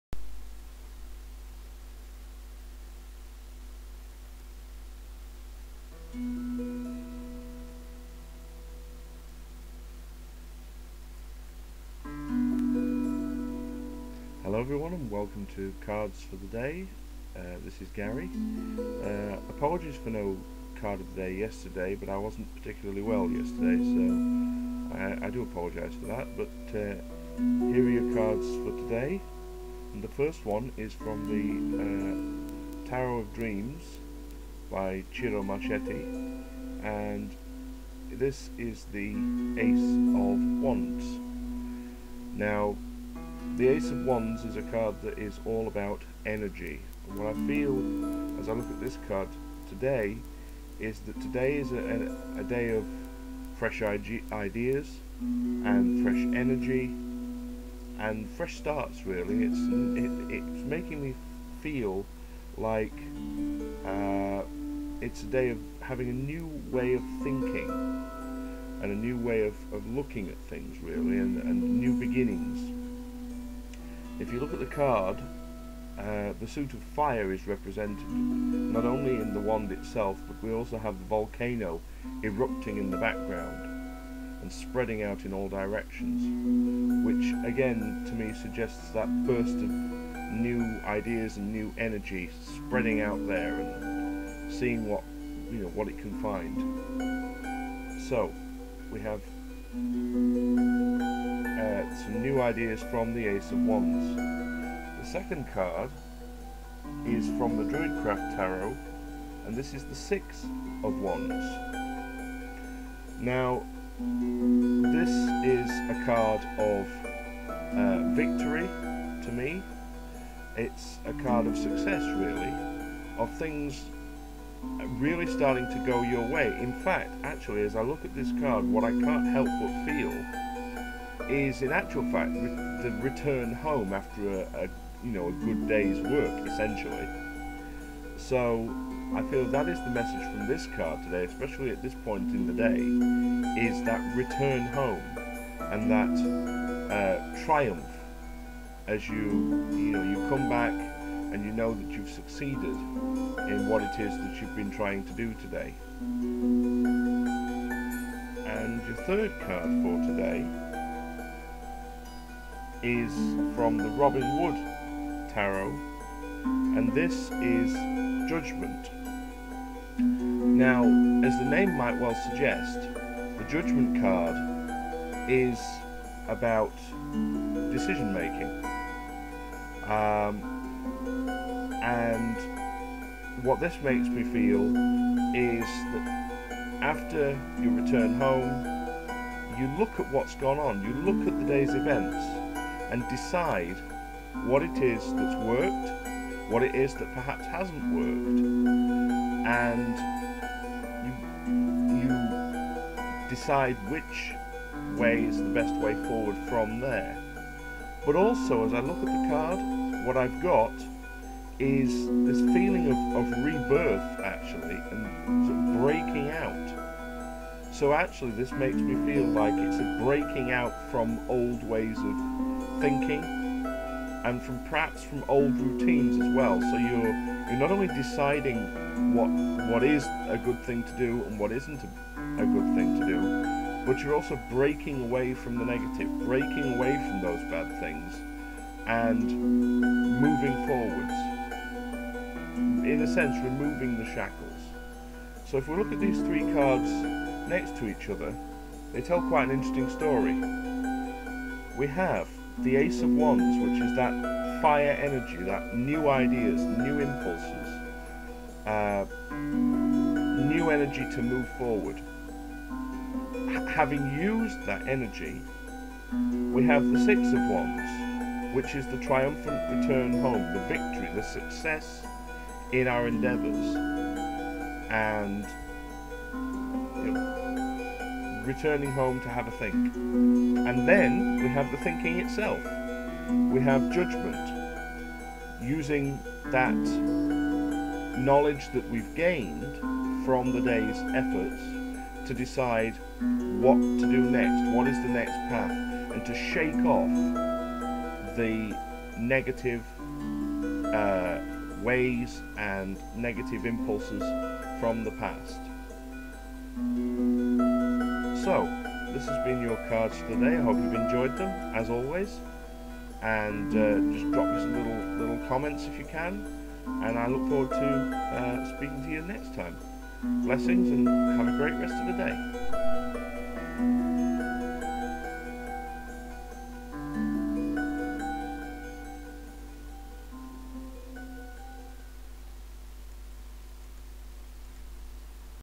Tarot reading "Card of the Day" - 17.03.12
Hi guys, apologies for no cards yesterday. I really wasn't feeling well and I still don't feel 100% myself today, but I wanted to try to do this for you. I hope you enjoy them.